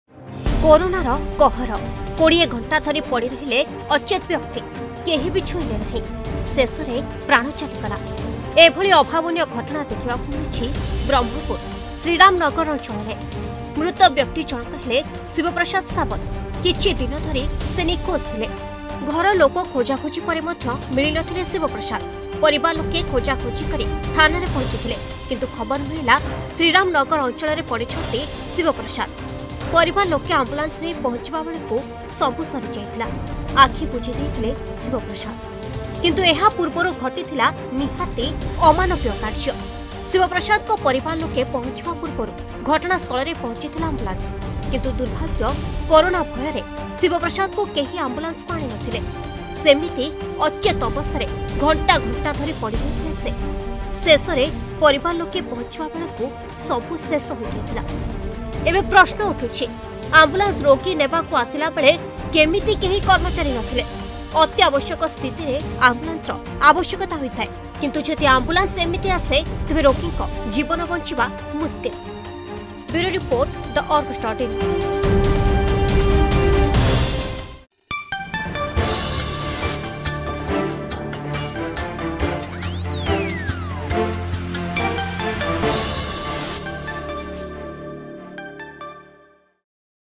କୋରୋନାର କହର , 20 ଘଣ୍ଟା ଧରି ପଡି ରହିଲେ ଅଚେତ ବ୍ୟକ୍ତି ...
କୋରୋନା ଭୟ ଏମିତି ଘାରିଛି ଯେ, ଜଣେ ଅଚେତ ବ୍ୟକ୍ତିଙ୍କୁ ଉଠାଇବାକୁ ଗଲେ ନାହିଁ ଲୋକେ । ଫଳରେ 20 ଘଣ୍ଟା ଧରି ପଡି ରହିବା ପରେ ପ୍ରାଣ ହରାଇଲେ ଅଚେତ ବ୍ୟକ୍ତି .